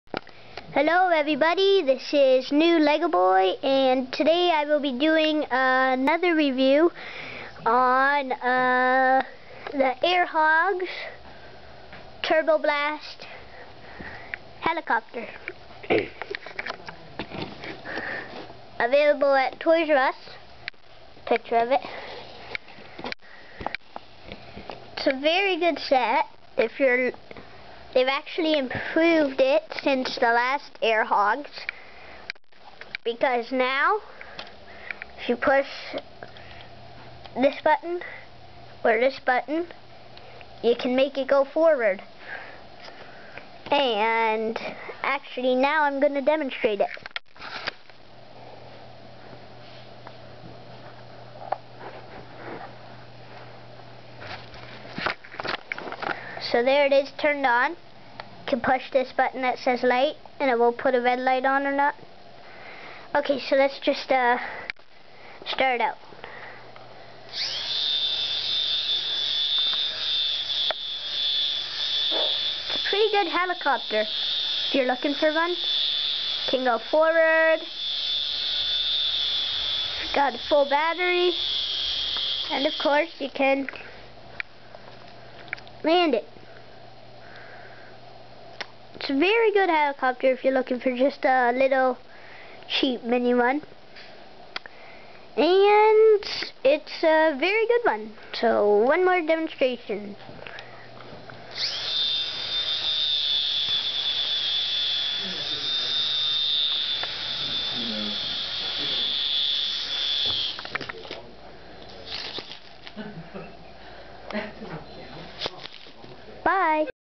Air Hogs Remote Controlled Helicopter (Update)
Here's the updated version of the old Air Hogs Helicopter!!! It is a really good helicopter because the update is that it lets you go forward now!!! Got it at toys r us.It is a really good miny helicopter if your looking for a small helicopter.     5/5 Stars